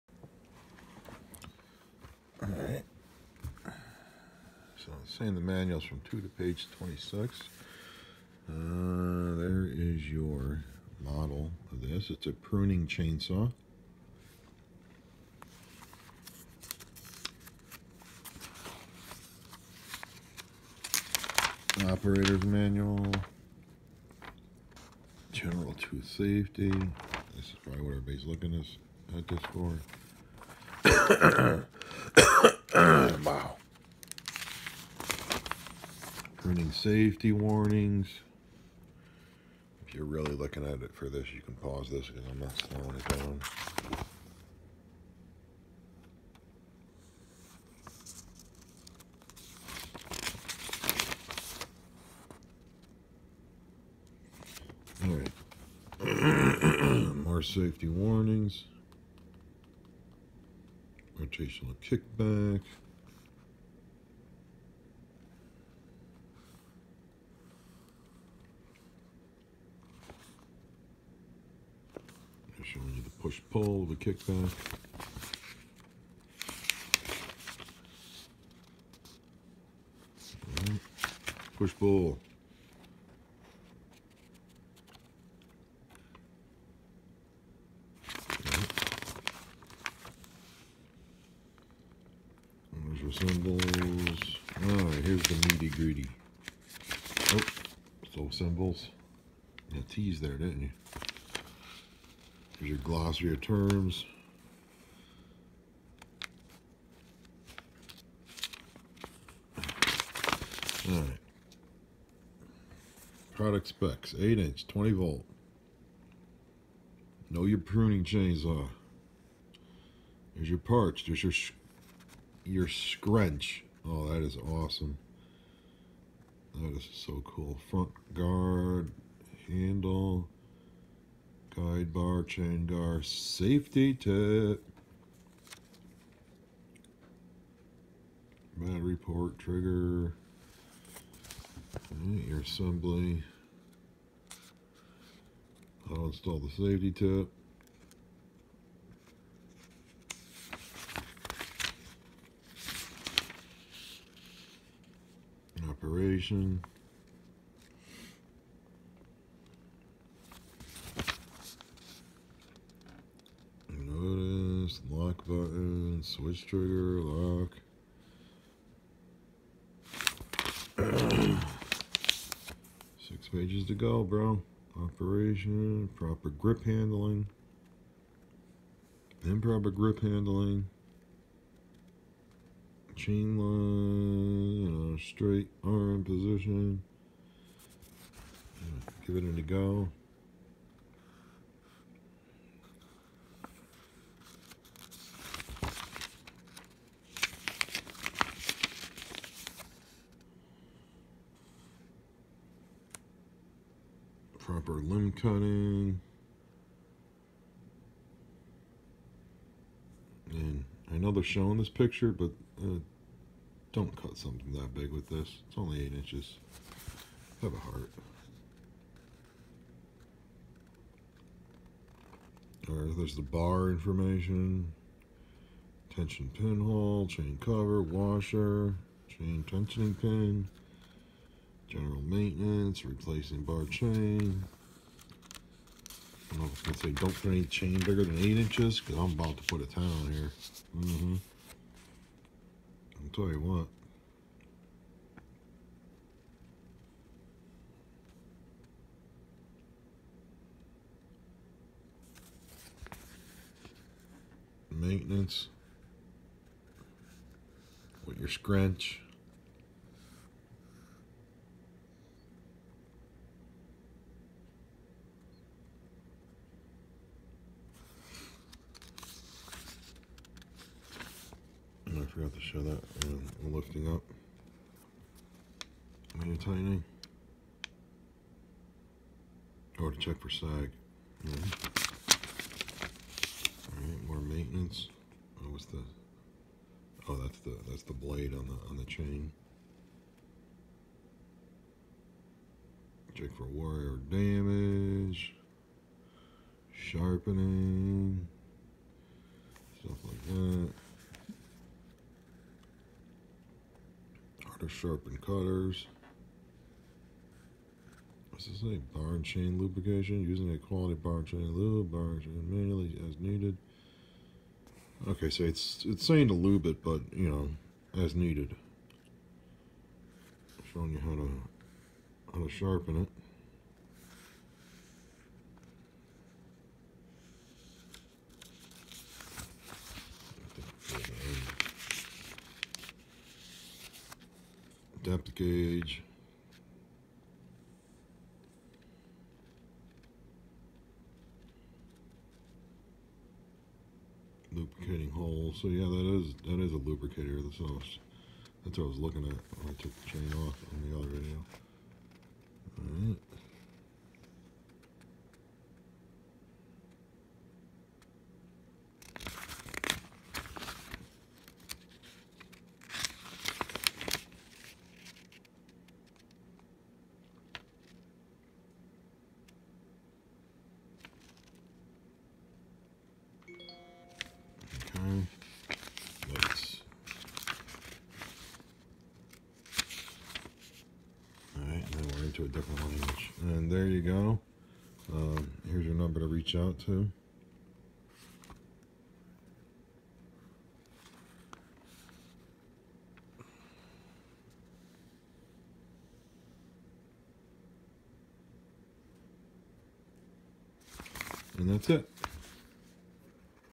Let's read the manual Hart HGCS01 20v Pruning Chain Saw 2ah Battery
Let's read the manual Hart HGCS01 20v Pruning Chain Saw 2ah Battery Charger
bob spurloc
bob spurlock